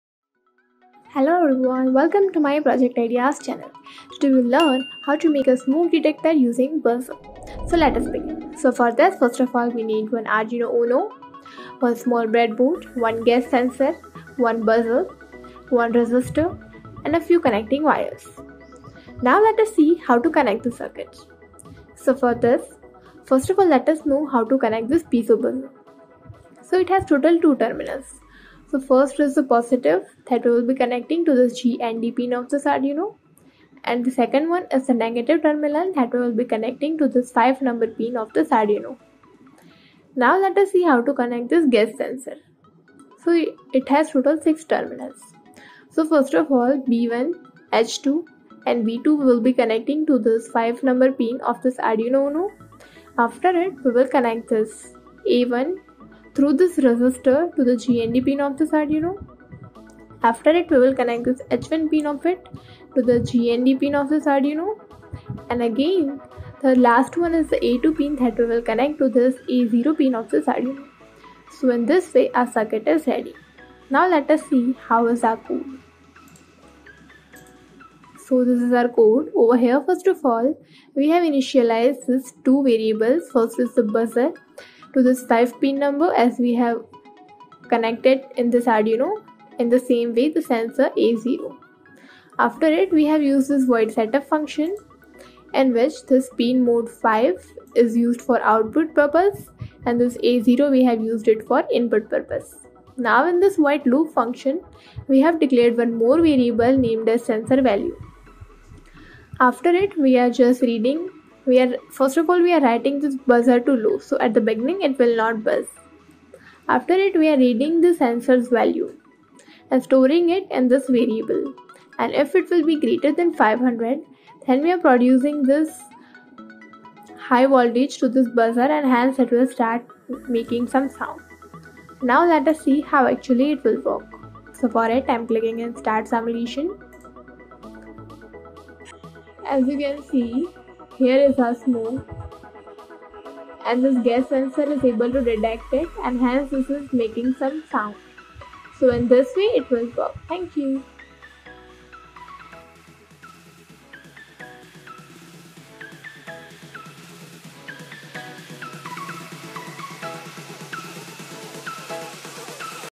Smoke Detector Using A Buzzer | Arduino Project
In this tutorial, we will make a Smoke Detector Using A Buzzer as part of our Arduino project.
To build this project, we will produce sound using a buzzer if the gas sensor detects the smoke.
This is a mini reflection of the fire safety system.

Are you looking for an exciting and practical DIY project that could help keep you and your loved ones safe? Look no further than building an Arduino Smoke Detector with Buzzer! In this DIY Arduino project, we will produce sound using a buzzer if the gas sensor detects the smoke. With easy-to-find components and our simple step-by-step instructions, you’ll be up and running in no time. This project is a mini reflection of the fire safety system.

Not only will you gain valuable knowledge about programming and circuitry, but you’ll also have the peace of mind that comes with knowing you’re prepared in case of an emergency. So what are you waiting for? Start building your Arduino Smoke Detector with Buzzer today, and be the envy of all your tech-savvy friends!

For physical projects, the components can be purchased online or from any electronics center.

For online projects, the TinkerCad website can be used.

Components:

To make a Smoke Detector using a Buzzer, we will require the following components:

● 1 Piezo Buzzer
● 1 Gas sensor
● 1 small BreadBoard
● 1 Resistor
● Connecting wires

Timestamp:

00:00 - Introduction
00:09 - Requirements
00:20 - Project Assembly
01:15 - Code Explanation
02:18 - Execution